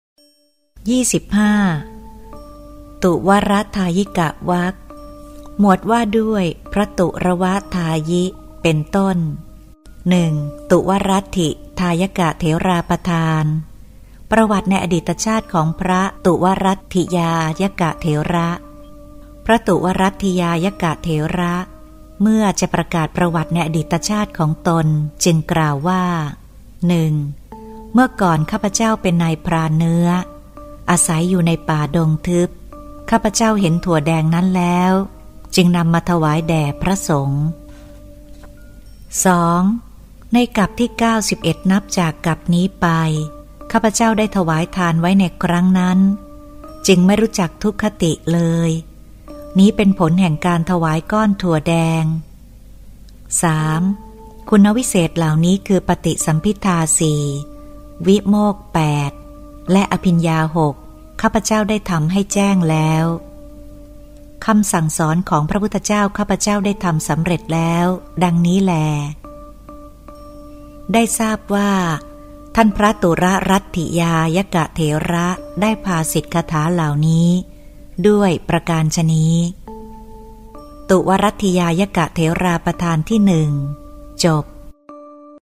32-243 ประวัติในอดีตชาติของพระตุวรัฏฐิทายกเถระ
๒๕. ตุวรทายิวรรค
            หมวดว่าด้วยพระตุวรทายิเป็นต้น
            ๑. ตุวรัฏฐิทายกเถราปทาน
         ประวัติในอดีตชาติของพระตุวรัฏฐิทายกเถระ
              (พระตุวรัฏฐิทายกเถระ    เมื่อจะประกาศประวัติในอดีตชาติของตน    จึงกล่าวว่า)
                            {๒๔๓}[๑]            เมื่อก่อน    ข้าพเจ้าเป็นนายพรานเนื้อ
                                      อาศัยอยู่ในป่าดงทึบ    ข้าพเจ้าเห็นถั่วแดงนั้นแล้ว
                                      จึงได้นำมาถวายแด่พระสงฆ์
                            [๒]            ในกัปที่    ๙๑    นับจากกัปนี้ไป
                                      ข้าพเจ้าได้ถวายทานไว้ในครั้งนั้น
                                      จึงไม่รู้จักทุคติเลย
                                      นี้เป็นผลแห่งการถวายก้อนถั่วแดง
                            [๓]            คุณวิเศษเหล่านี้    คือ    ปฏิสัมภิทา    ๔    วิโมกข์    ๘
                                      และอภิญญา    ๖    ข้าพเจ้าได้ทำให้แจ้งแล้ว
                                      คำสั่งสอนของพระพุทธเจ้า    ข้าพเจ้าก็ได้ทำสำเร็จแล้ว    ดังนี้แล
            ได้ทราบว่า    ท่านพระตุวรัฏฐิทายกเถระได้ภาษิตคาถาเหล่านี้    ด้วยประการฉะนี้
            ตุวรัฏฐิทายกเถราปทานที่ ๑ จบ